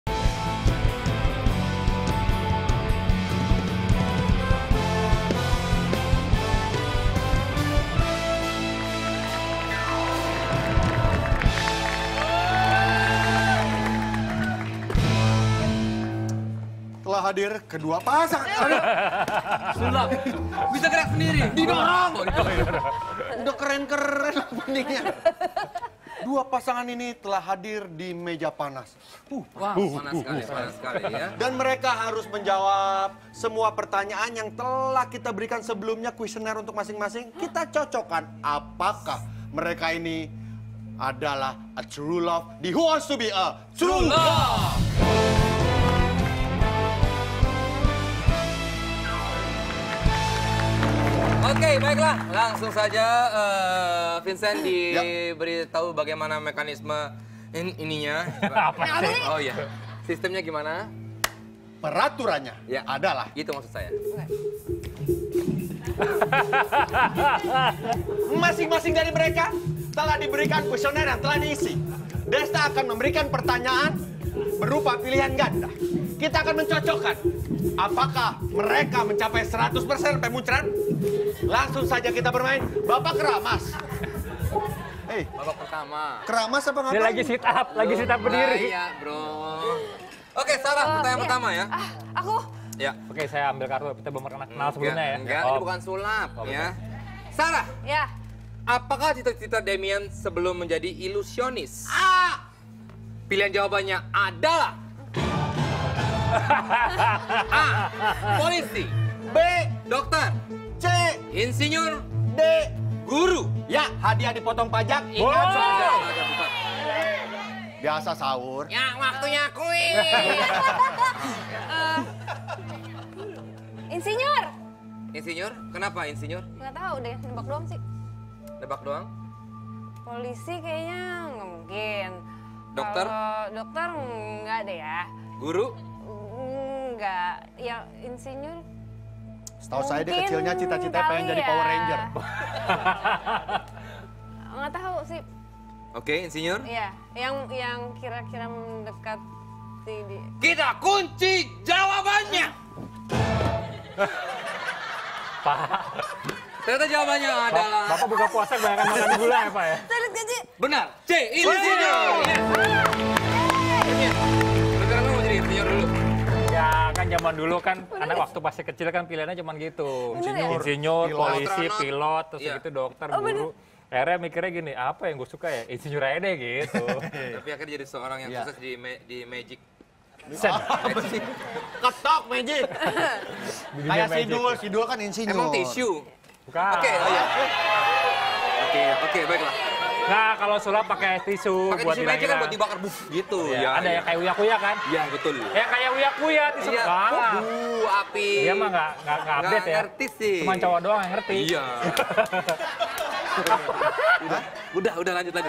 Hebaaaaatttt! Sara Wijayanto Bisa Tebak Cita-cita Masa Kecil Demian
Download Aplikasi Zulu di Google Play & App Store